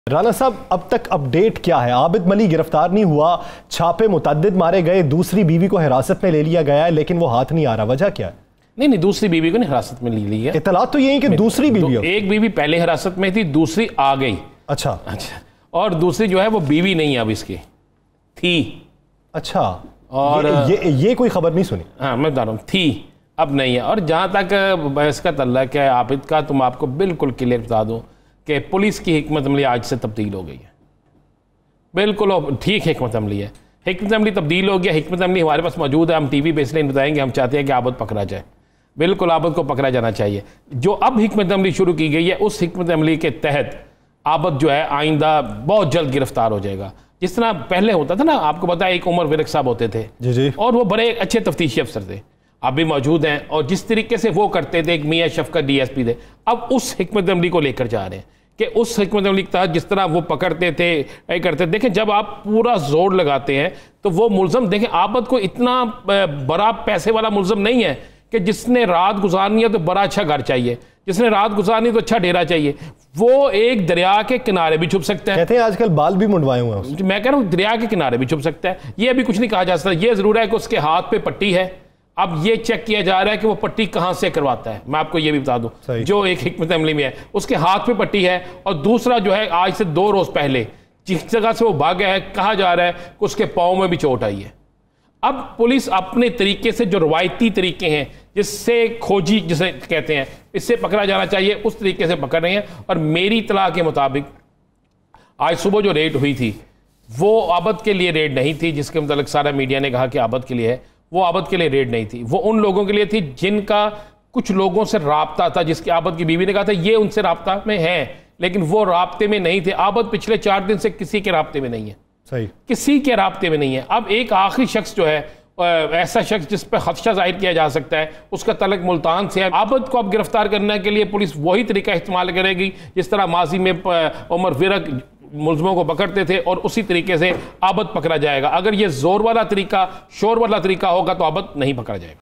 Rana Azeem assures public about Abid Malhi arrest | 92NewsHD
Rana Azeem assures public about Abid Malhi arrest | 17 September 2020 | 92NewsHD